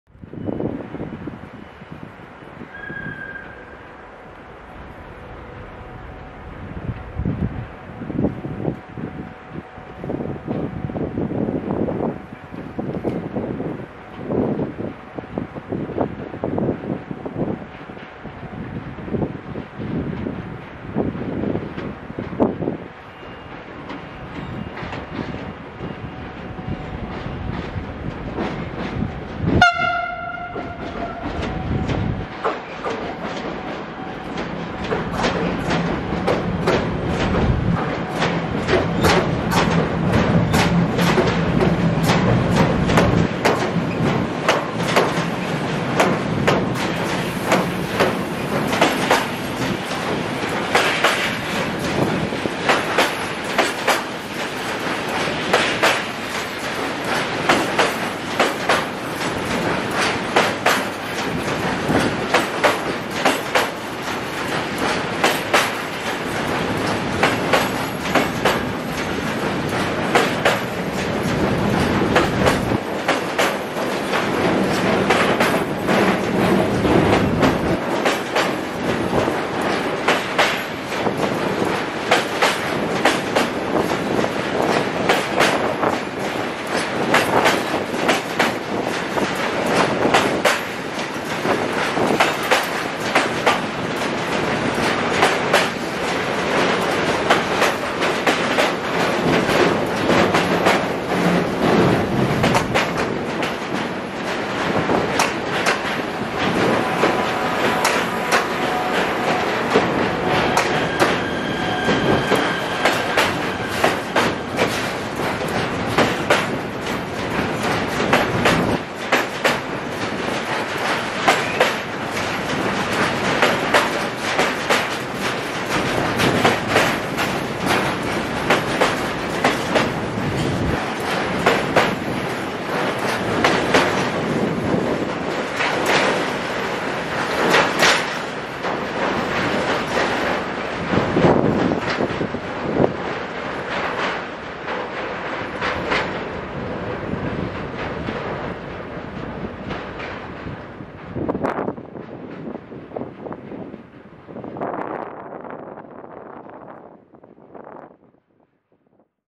Влакче на "PIMK Rail" минава през Централна гара София с великолепните 80 001 и 80 019...
🟢 Freight train of "PIMK Rail" passes through the Central Railway Station Sofia on 28-March-2022, driven by the locomotives Siemens Smartron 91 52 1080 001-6 and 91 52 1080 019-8 "Irina"...